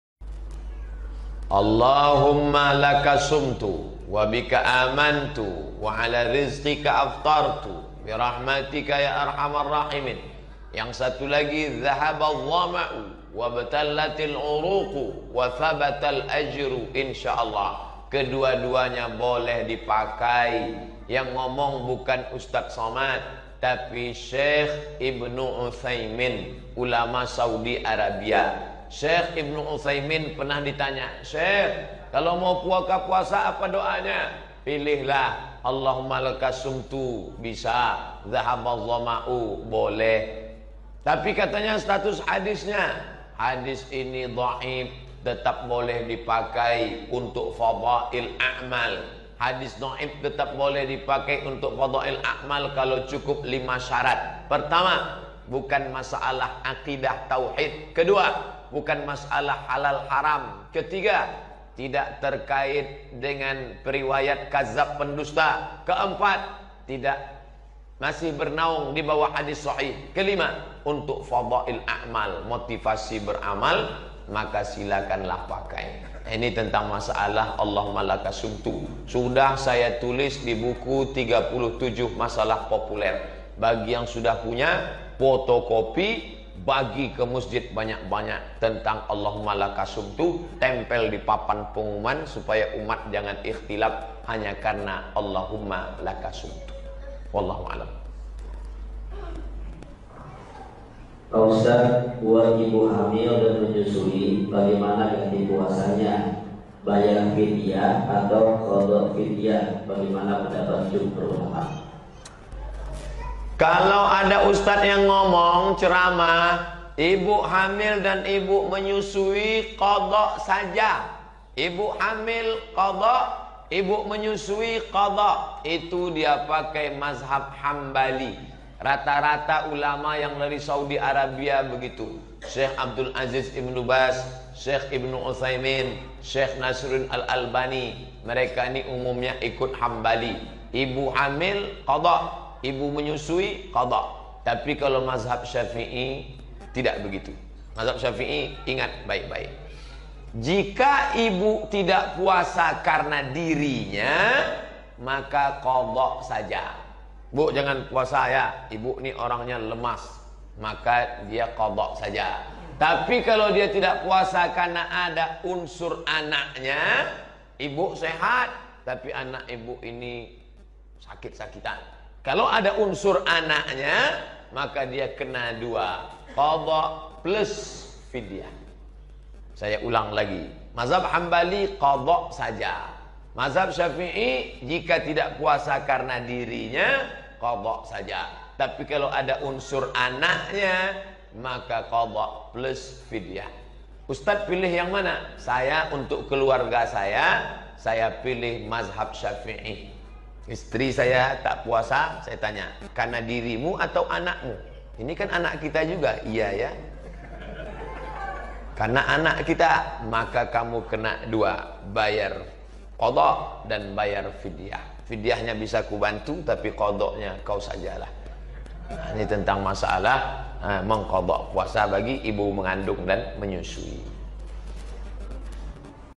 Allahumma lakasumtu - TIDAK SHOHIH/DHOIF????? USTADZ ABDUSSAMAD
Semoga bulan ramadhan ini semua amal ibadah kita di terima dan di lipat gandakan oleh Allah SWT